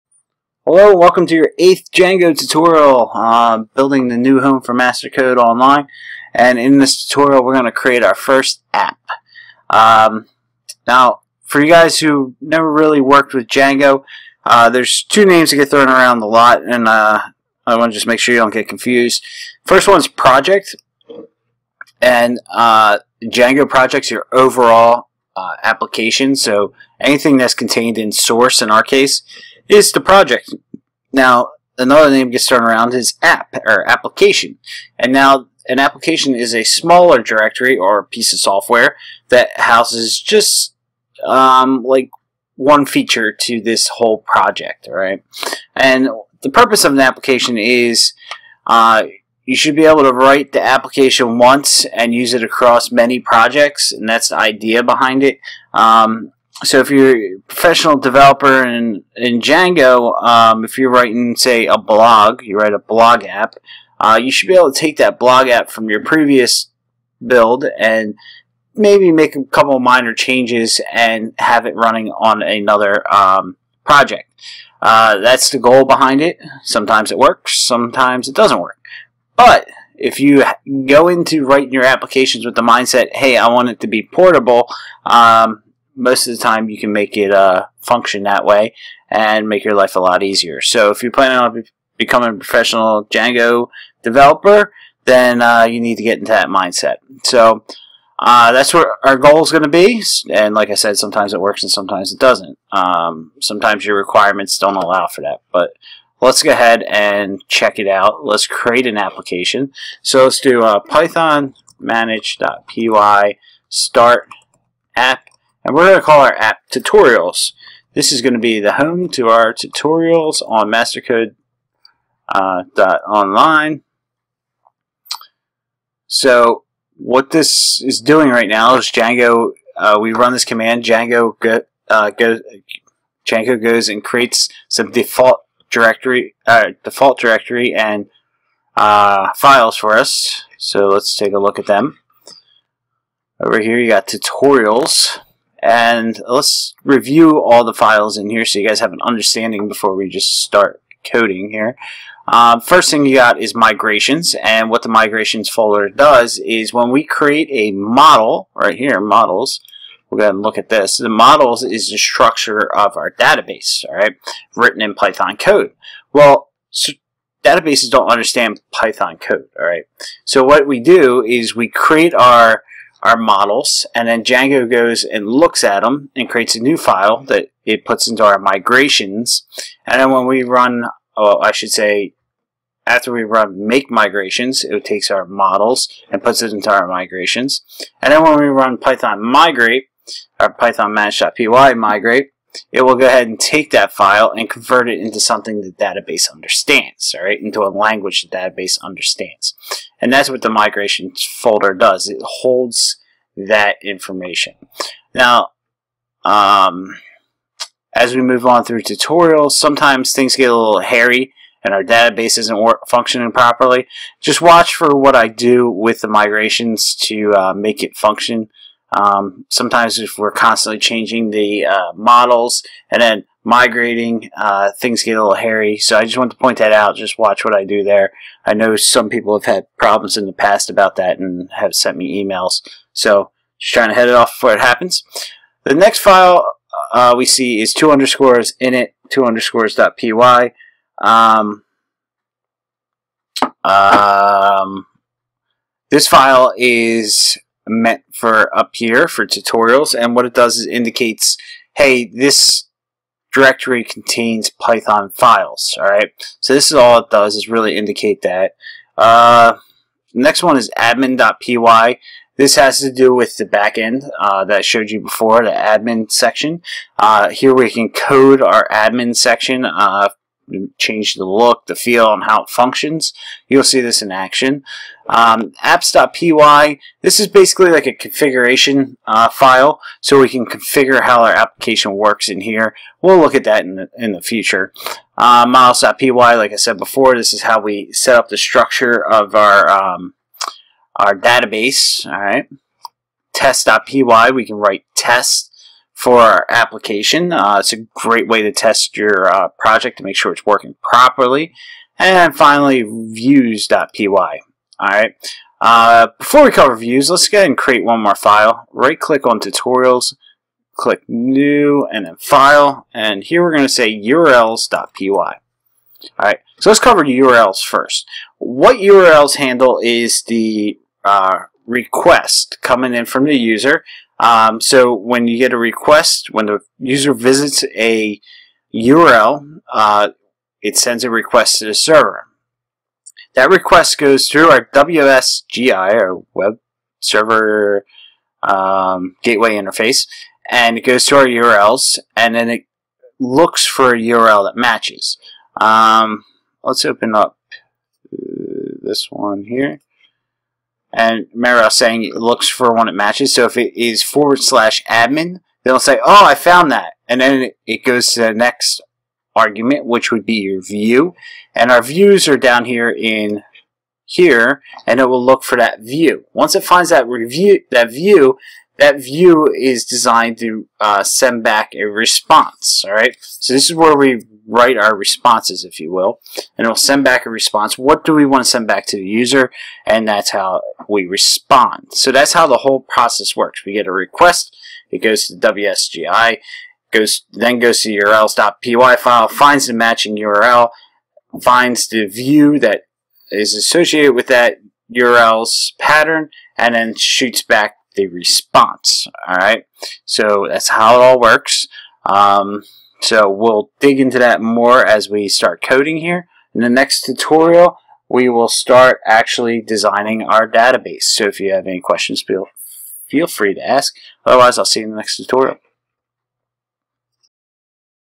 Django Tutorial - Create Our First Django Application
In this Django tutorial, we create our first app for our Django Project. We will also review the default files that Django includes for us.